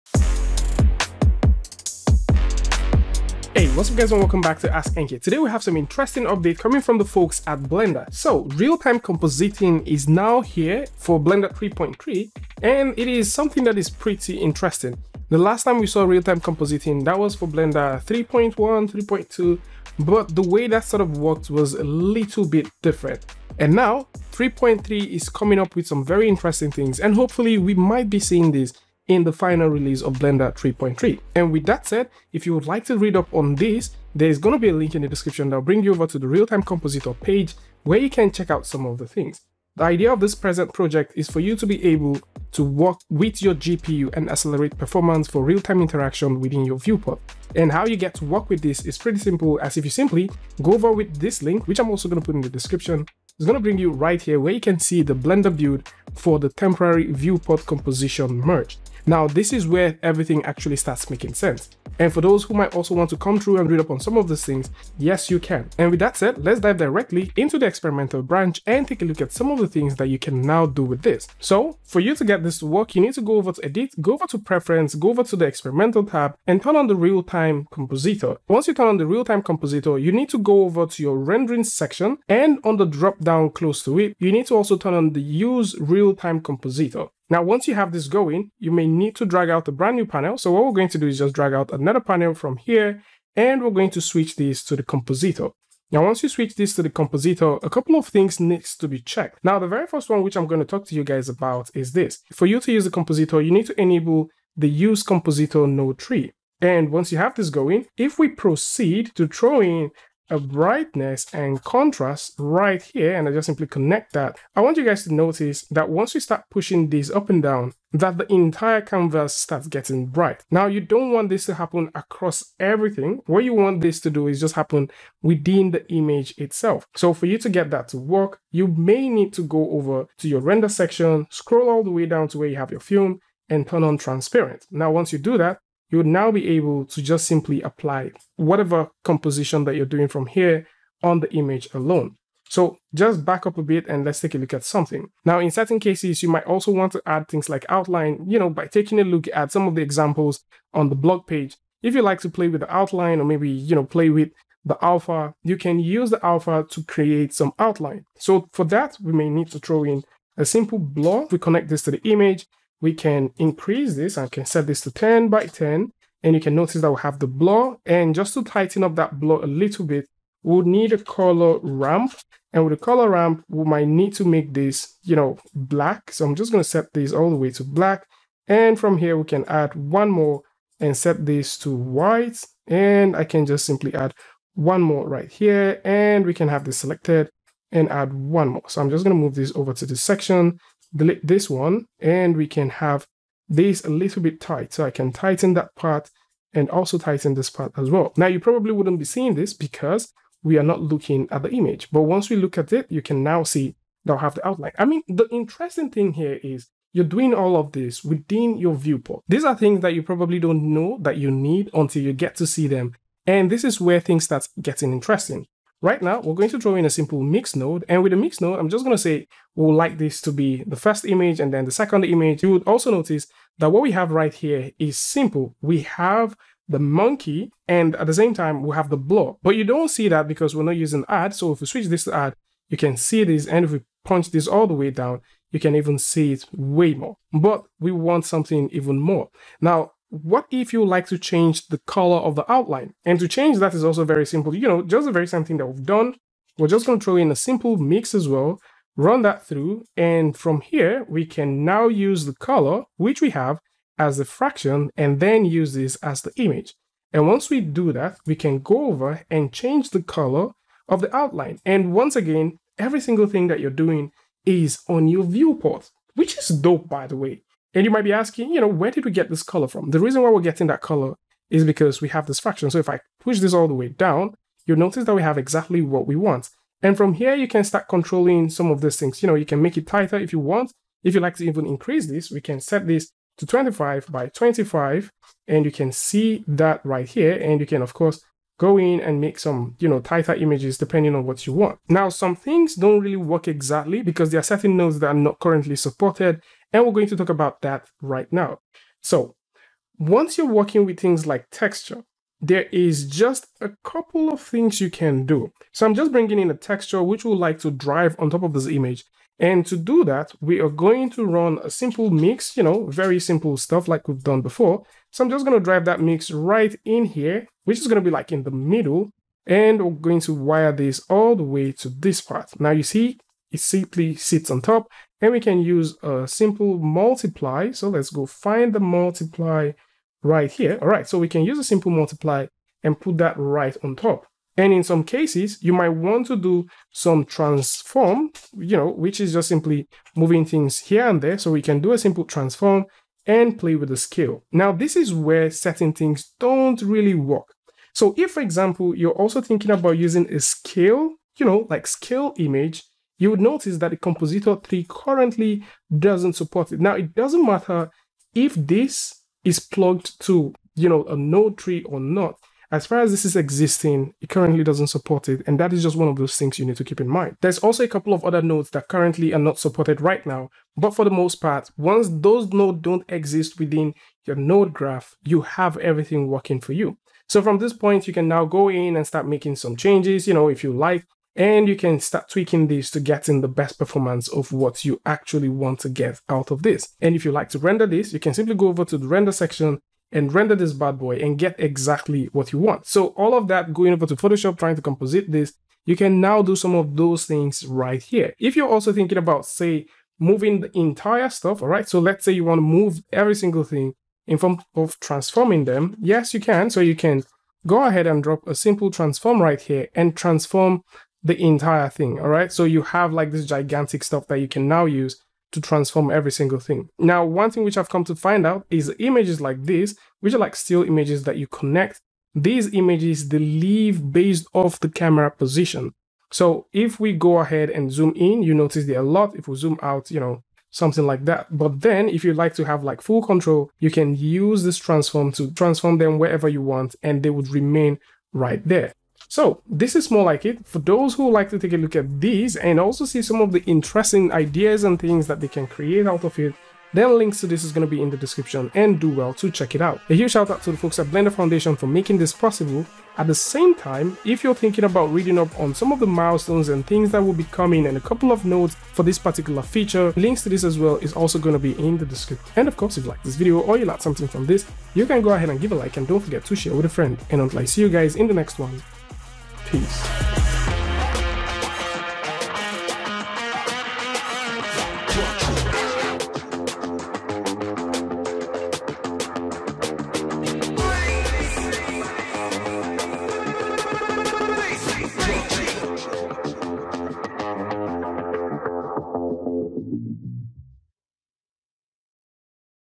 Blender Is Getting More Awesome!
Realtime compositing is now in a blender and it looks awesome!

Car Traffiq Addon 1.5 (33%)

00:00 Intro
01:12: Getting Started
01:50 Exploring Realtime
03:32 More Cool Stuff
05:00 Texture+
05:48 Bugs
06:29 More Talks
07:19 Final Words